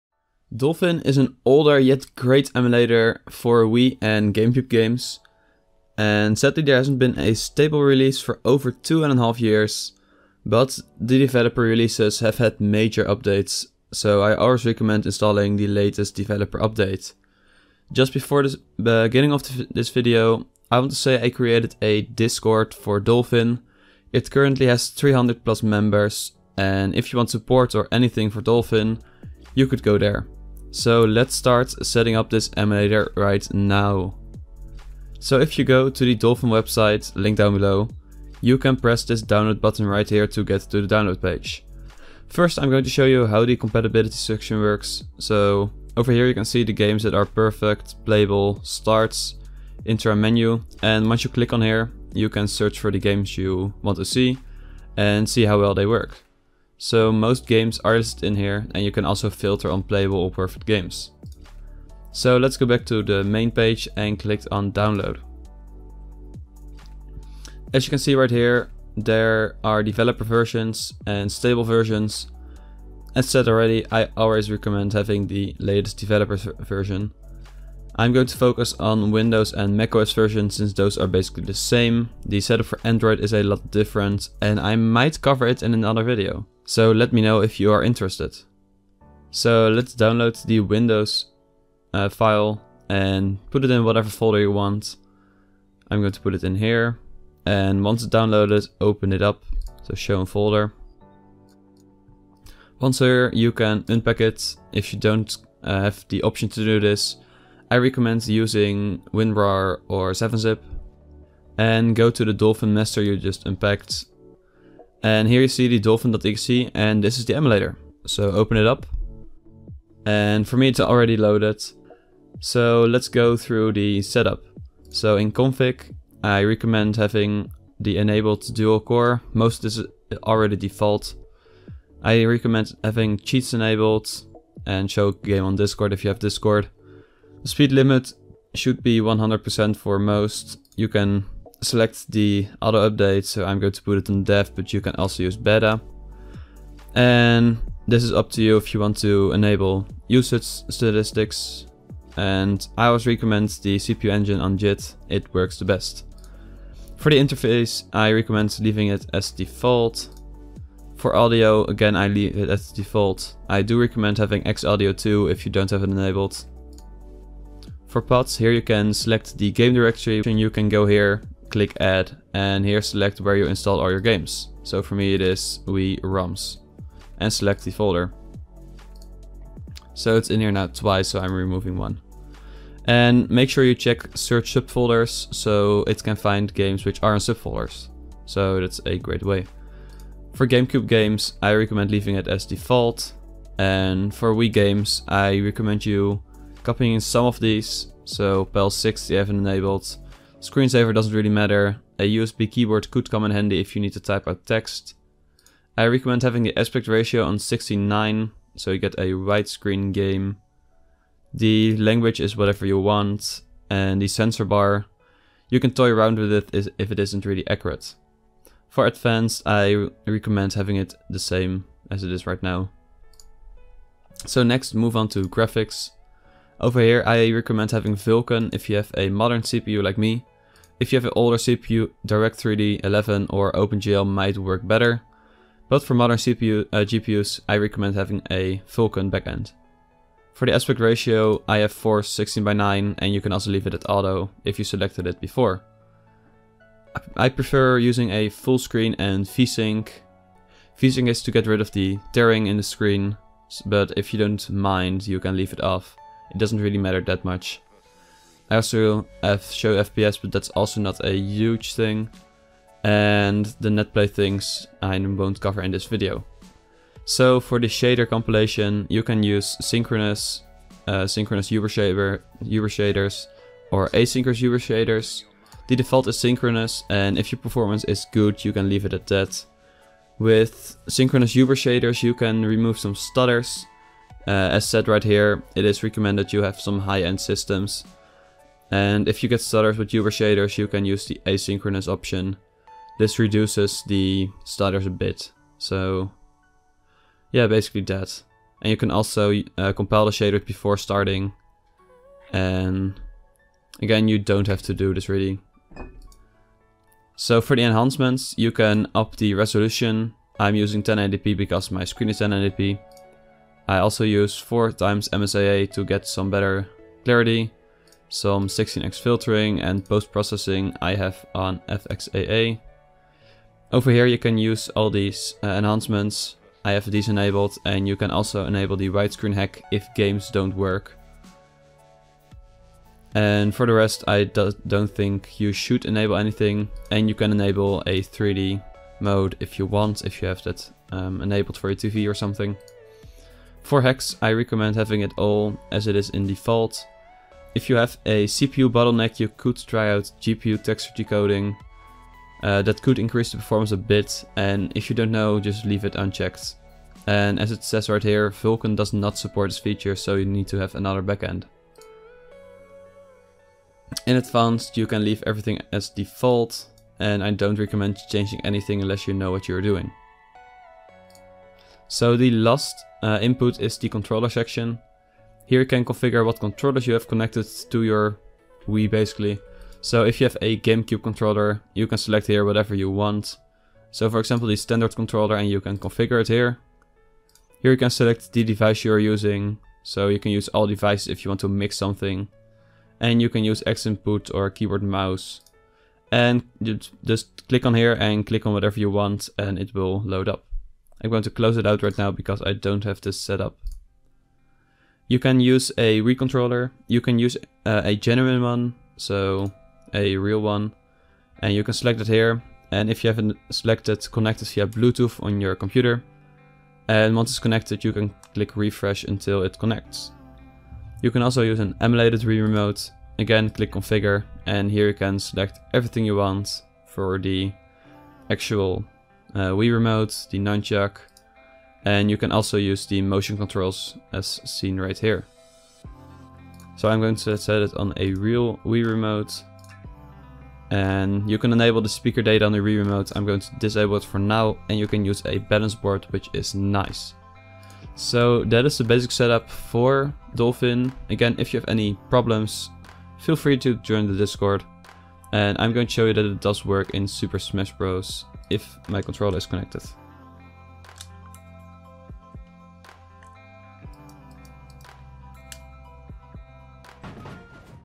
How To Install the Dolphin Wii and GameCube Emulator!?
Dolphin is a popular emulator for the Wii and GameCube. With it's age it has been updated a lot, to made most games run perfectly on desktops! This guide works for Windows and Linux. For Android and Linux you should look for another guide (:
To find out how to install this emulator, check the full video!

Timestamps:
0:34 Where to download Dolphin + compatibility page
2:03 Basic configurations
3:45 Graphics configuration
6:12 Controller configuration

My PC specs:
GTX 1080 FE
16 GB DDR4 3000MHz
Asus B350 Prime Plus